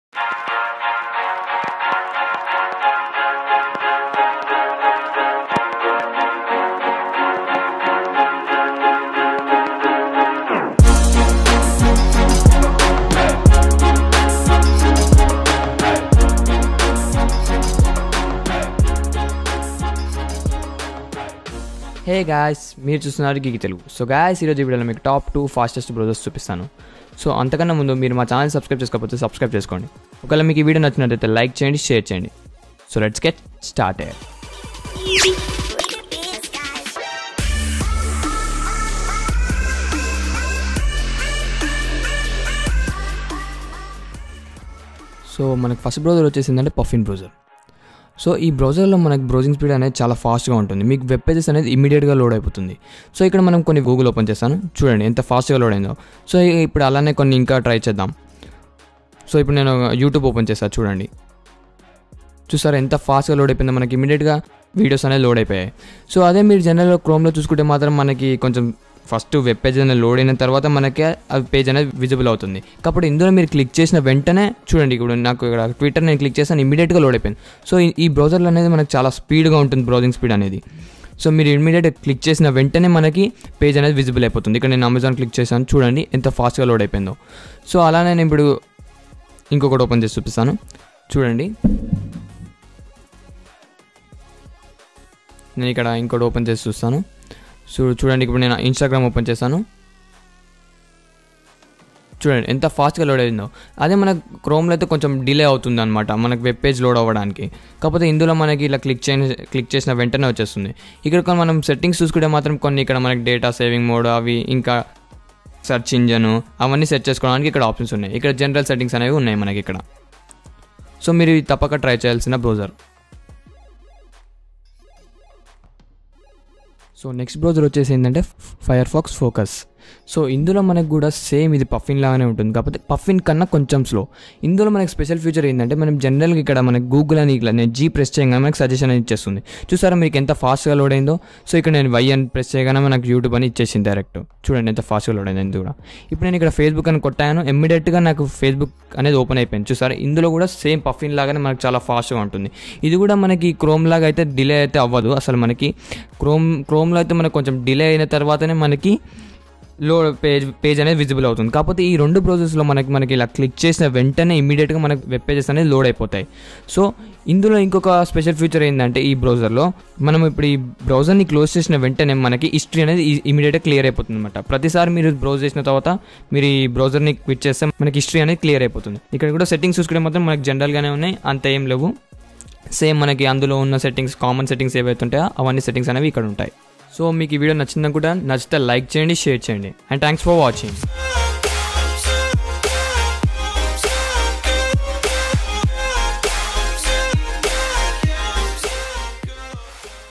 Top 2 Fastest Browsers For Android | Telugu | Geeky Telugu
Top 2 Fastest Browsers For Android

These are the top 2 fastest browser for android that you must try, to find out how much fast there are watch this video.

NOTE : All content used is copyright to Geeky Telugu ,